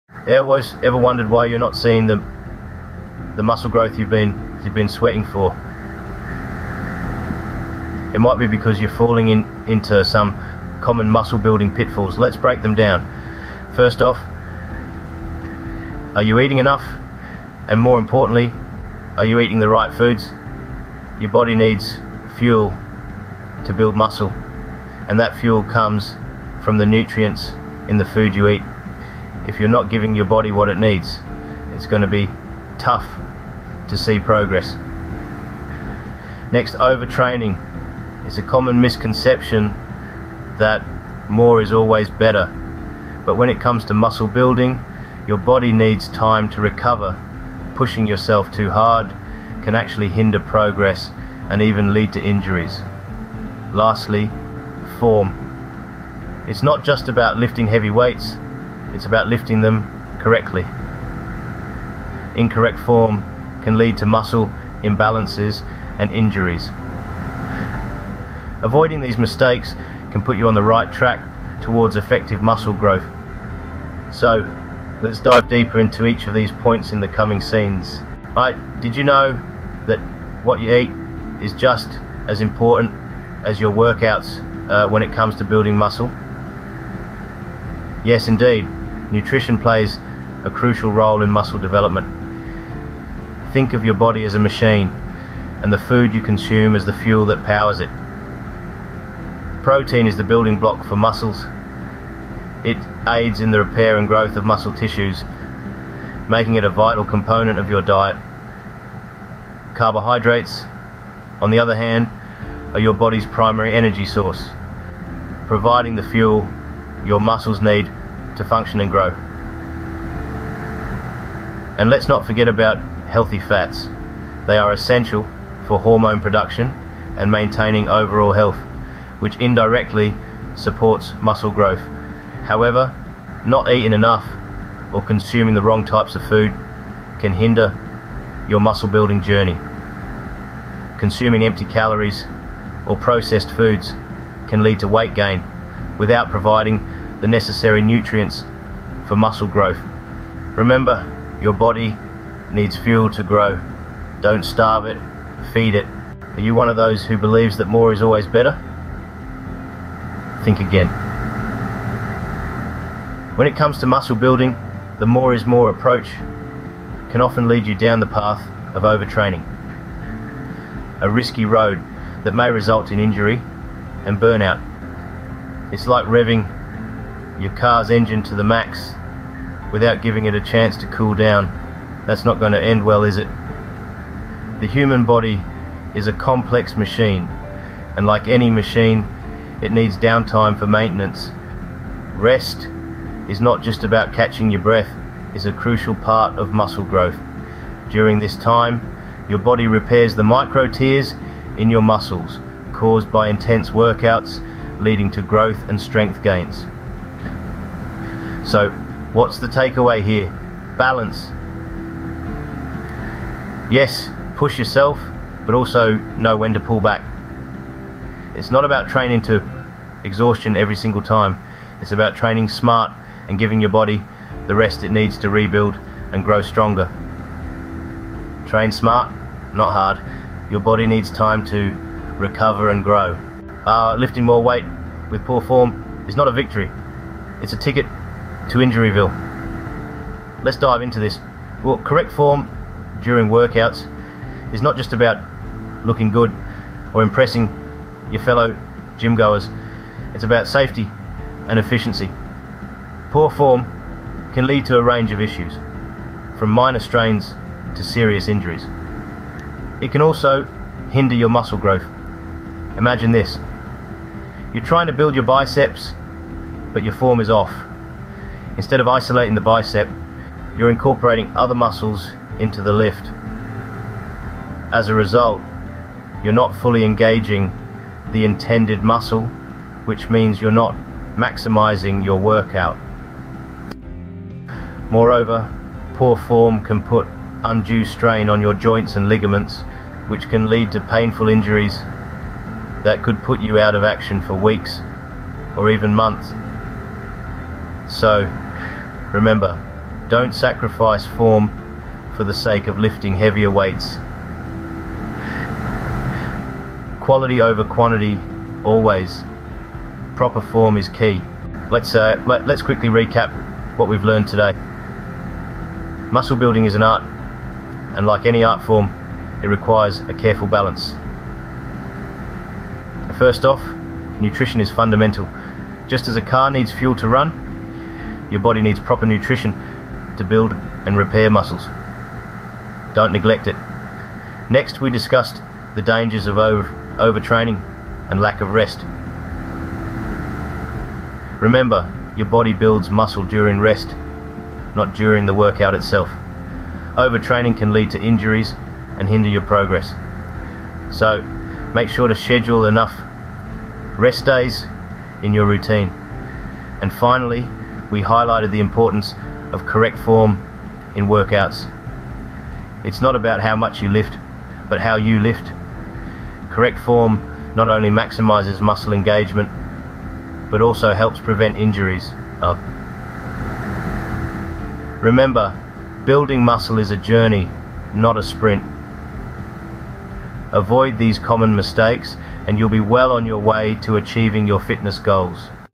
Muscle Building Mistakes to Avoid Common Pitfalls
Are you struggling to see the muscle-building results you desire? In this video, we'll dive into the common pitfalls that may be hindering your progress in the gym. From training techniques to nutrition and recovery, we'll cover the crucial mistakes to avoid on your journey to building lean muscle.

🏋️‍♂️ **Training Techniques**: Learn about the most common mistakes people make in their workout routines that can stall muscle growth and how to correct them for optimal gains.

🍲 **Nutrition Blunders**: Discover the dietary missteps that could be holding you back from packing on muscle mass, and get expert tips on crafting a muscle-building diet.

😴 **Recovery Roadblocks**: Find out how inadequate recovery can impede your muscle-building efforts and how to maximize your recovery for enhanced results.

Join us as we unravel the muscle-building mistakes you need to steer clear of, and take your physique to the next level! Let's crush those fitness goals together! 🏋️‍♀️✨

00:00:00 Common Muscle-Building Mistakes
00:01:18 The Importance of Nutrition
00:02:36 Overtraining and Lack of Rest
00:03:53 The Importance of Correct Form
00:05:11 Summary of Key Points